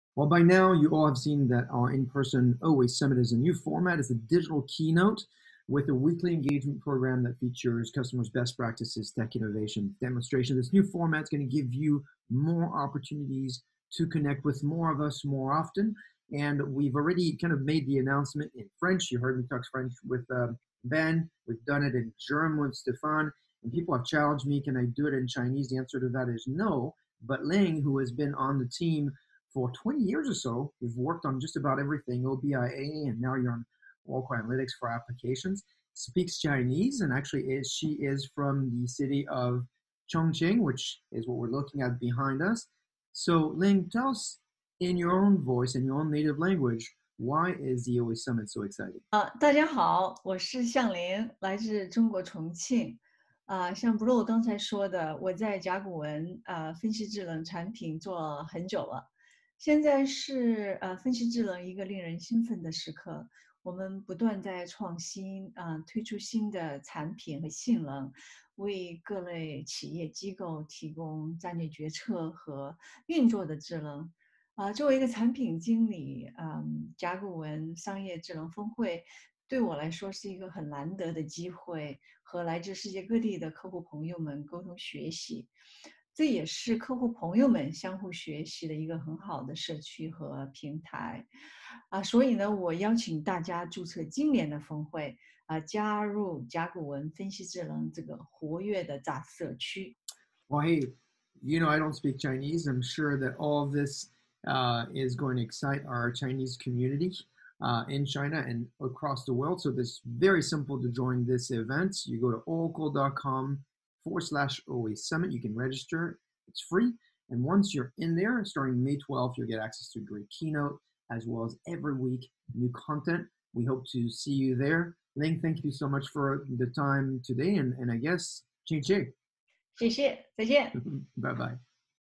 The Oracle Analytics Summit...in Chinese!
Our OASummit is truly going global! This week, Ling Xiang 向凌 of the OracleAnalytics for Application team tells us in Chinese why you should join our great community event!